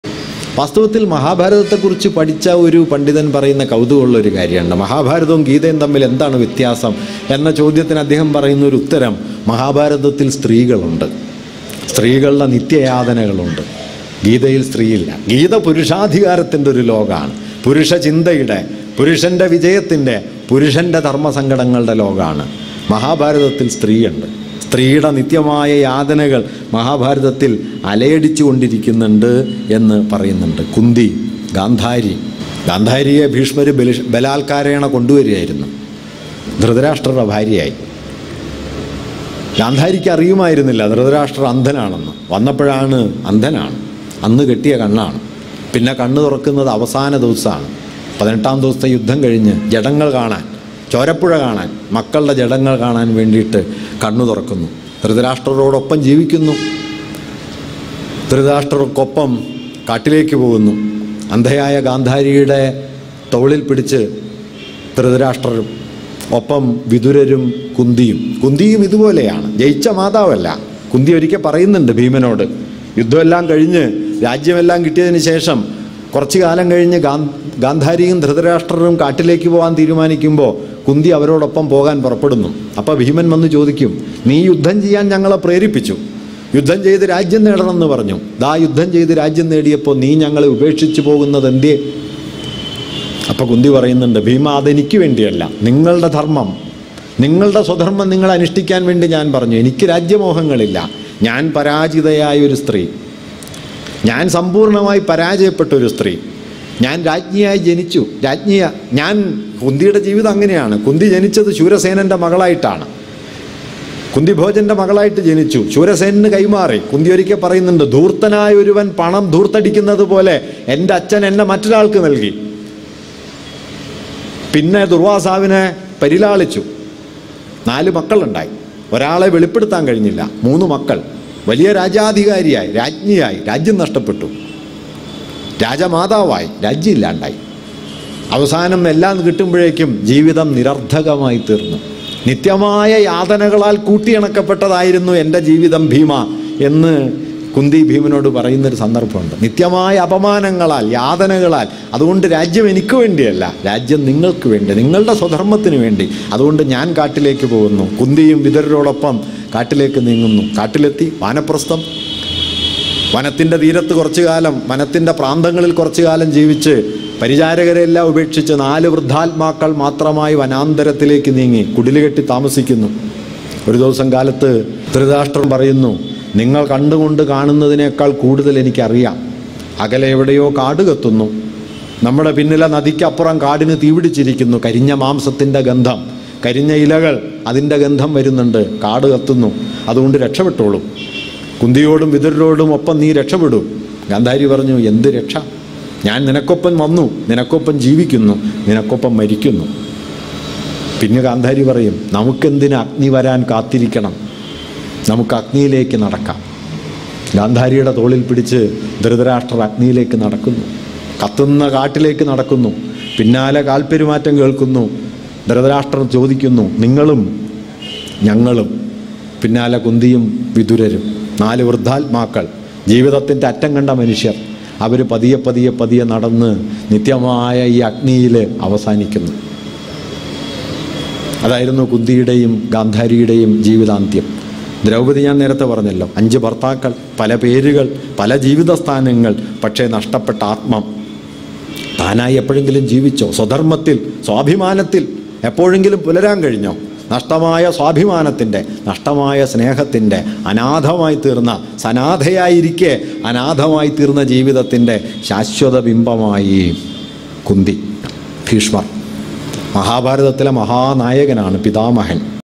മഹാഭാരതത്തിലെ മൂന്നു സ്ത്രീകൾ l സുനിൽ പി ഇളയിടം-By Sunil P Ilayidam
Dr. Suni P Ilayidom on Bheeshmar.
SPEECH BY SUNIL P ILAYIDOM